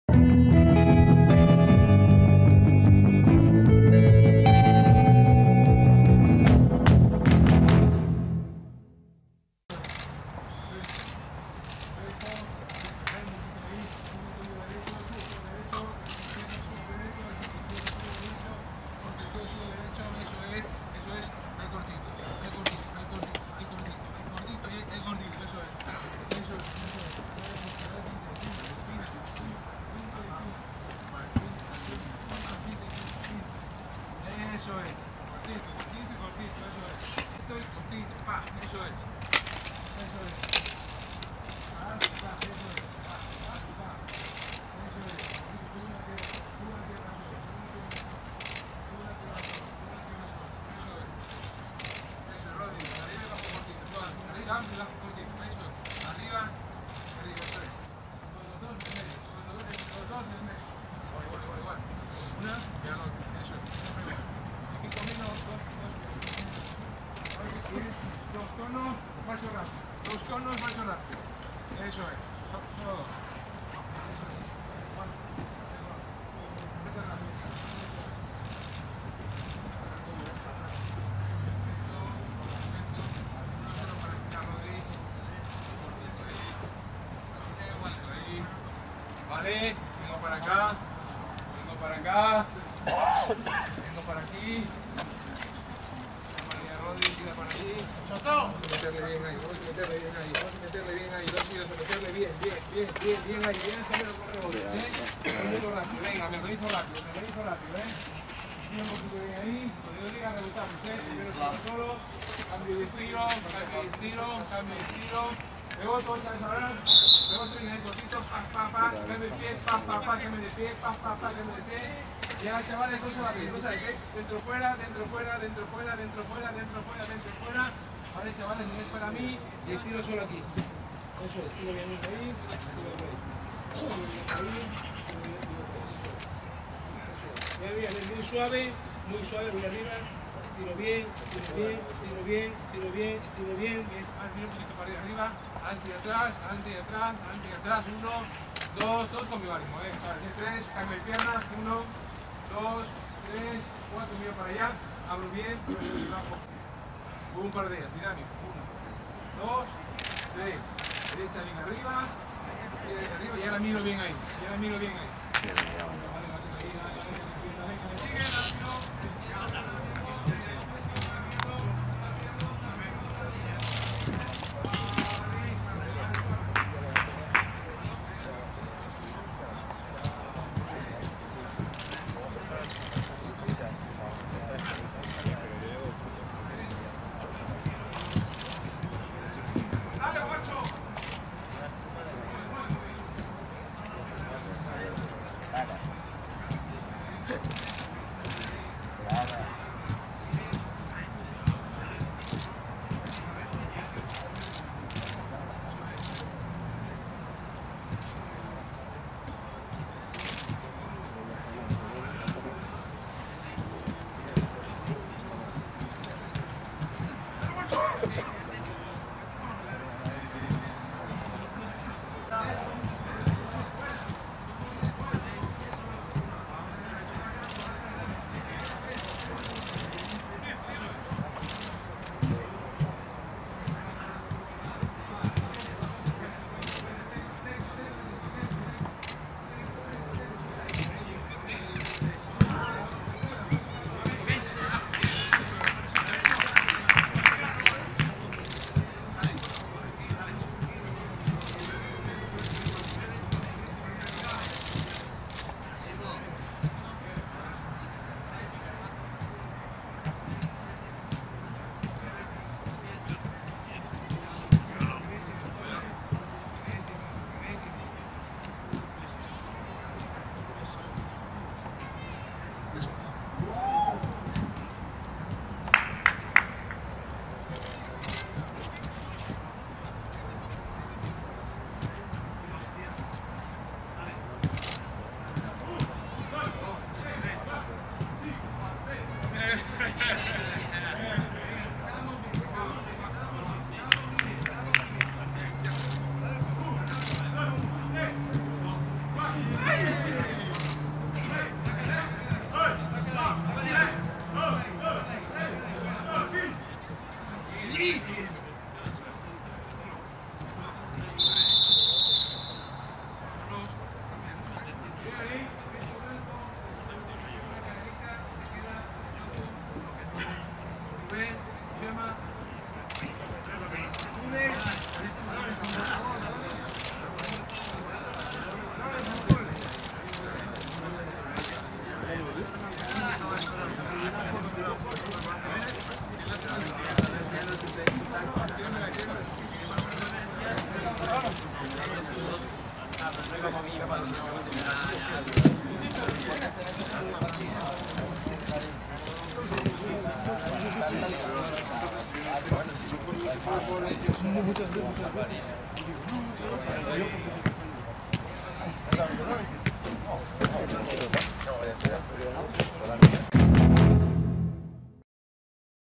Entrenamiento 31 de diciembre
El Valencia CF ha cerrado el año 2017 con el último entrenamiento en la Ciudad Deportiva de Paterna. La gran ausencia, un día más, ha sido la del entrenador Marcelino García Toral. Tampoco  se han ejercitado con el grupo Carlos Soler y Murillo que siguen recuperándose de sus lesiones.